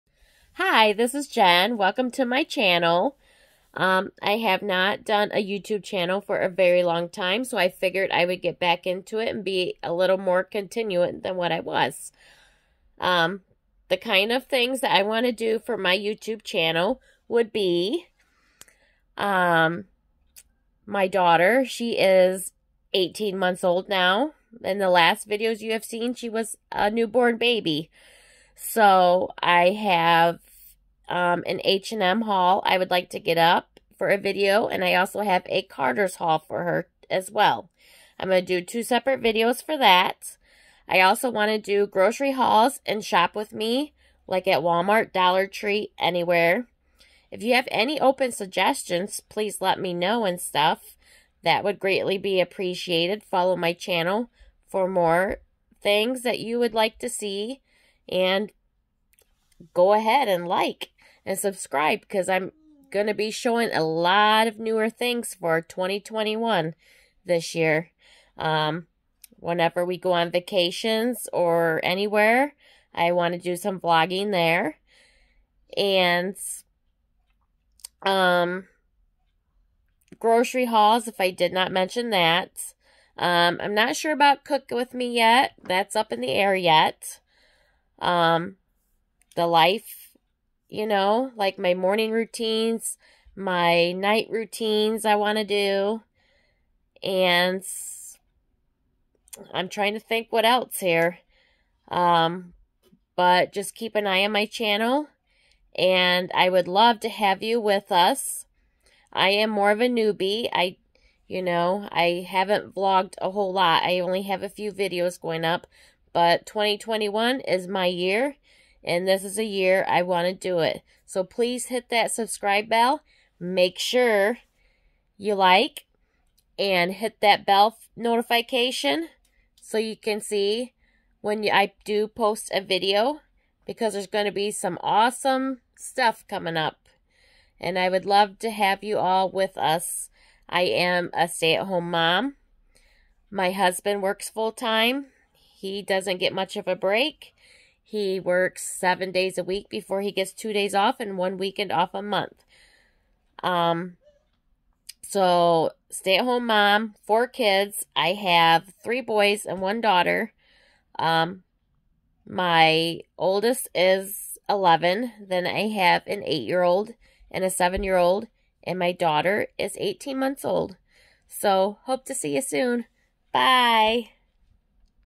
Keeping up with 2021
This year for 2021 I want to be more consistent with my vlogs.

My channel will have the following

morning and night time routines.
grocery hauls.
kids hauls and other hauls.

And possibly some other ideas in mind also.

If you would like to correspond with me

We love letters
Jennifer Hammon
31 Sunnydale Lane
Napoleon, Ohio 43545

Please, bare with me as I am still new to all of this. And it's been quite awhile. Until I figure how to edit more and all.